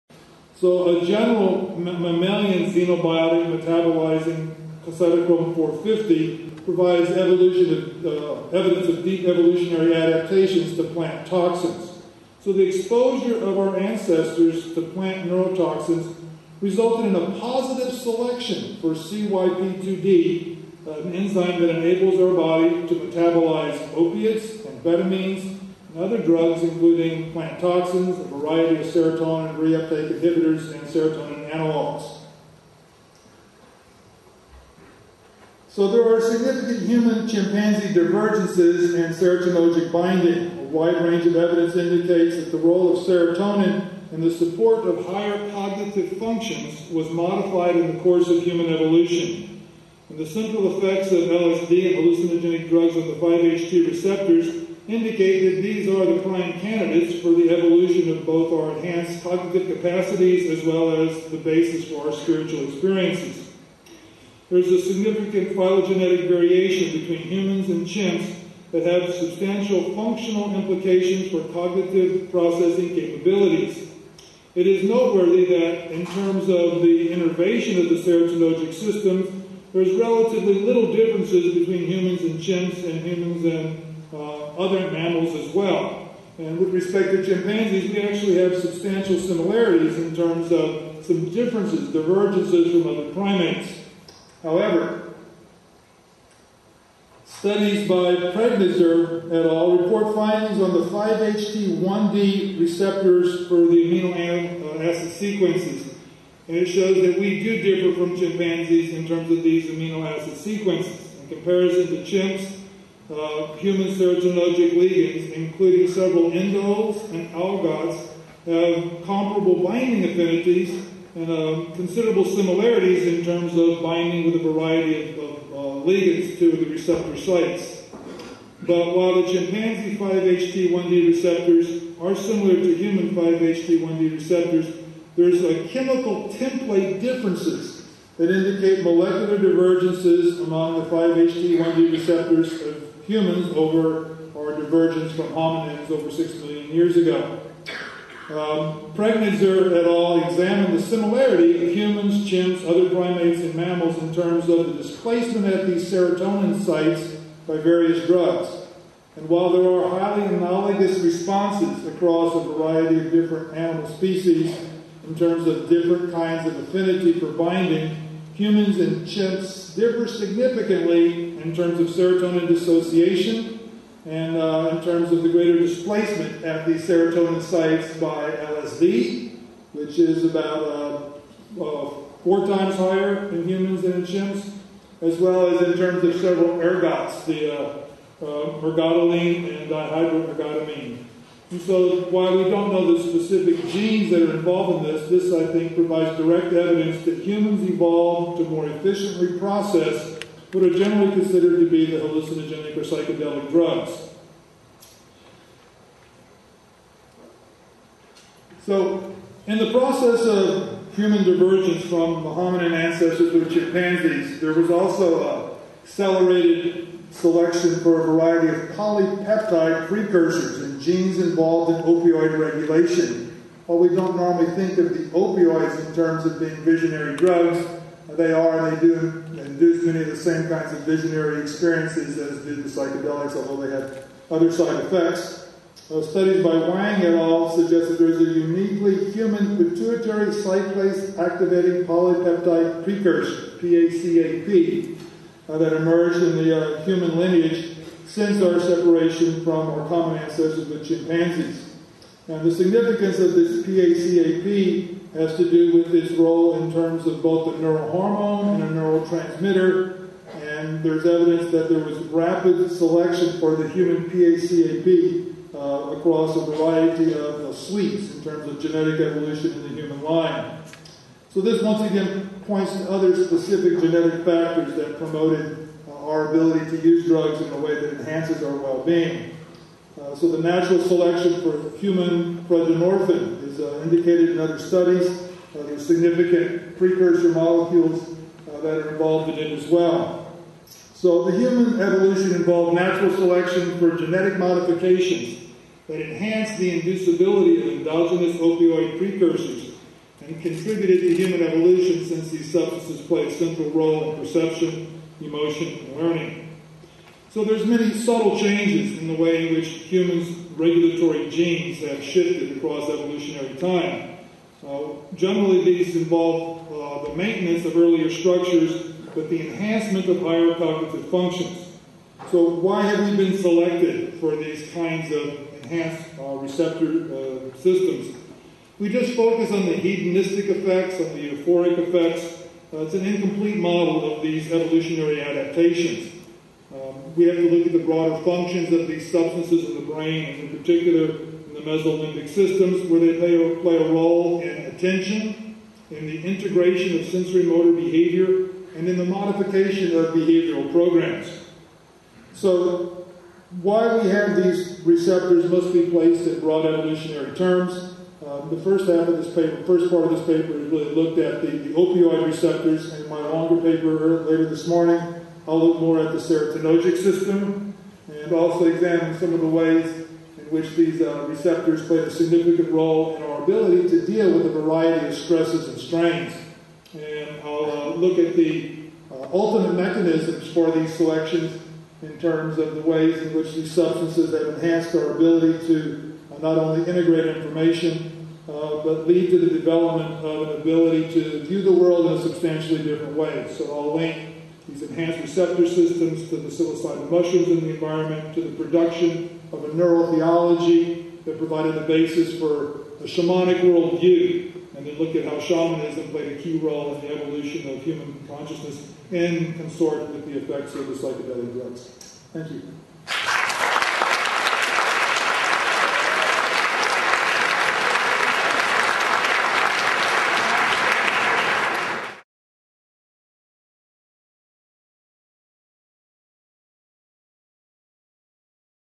M.J. Winkelman at the World Psychedelic Forum[part 2 of 2]
M.J. Winkelman about psychedelics and human evolution.